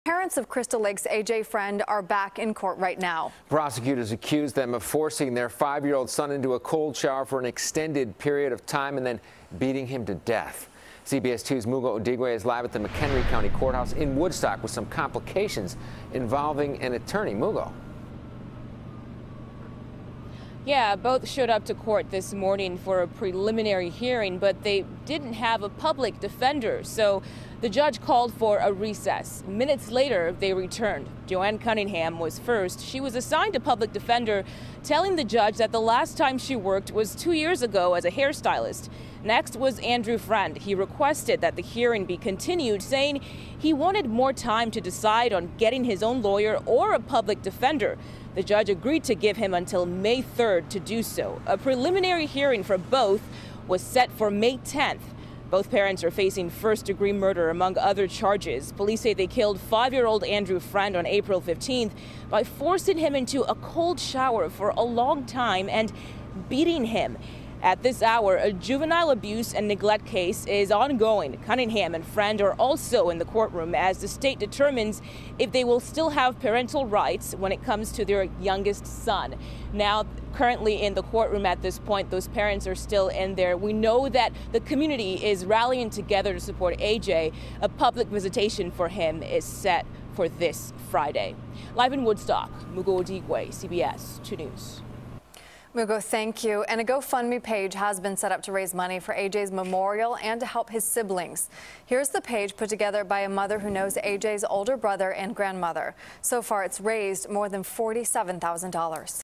A.J. Freund's Parents Face Hearing On Murder Charges
A McHenry County judge has assigned a public defender to represent the mother of 5-year-old A.J. Freund against murder charges, but his father is seeking more time to decide if he also wants a public defender, or to hire his own attorney. CBS 2's Mugo Odigwe reports.